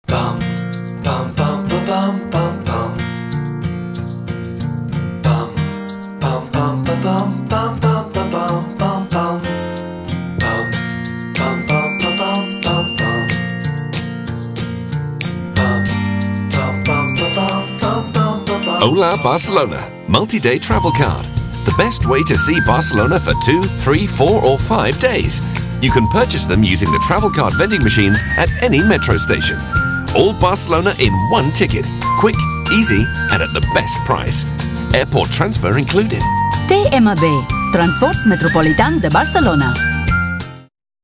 Hola BCN!
Campanya de comunicació dels títols de transport Hola Bcn! La nova marca creada per TMB per a turistes i visitants perquè es puguin desplaçar per la ciutat i l'àrea metropolitana en el transport públic regular, sense límit de viatges durant dos, tres, quatre o cinc dies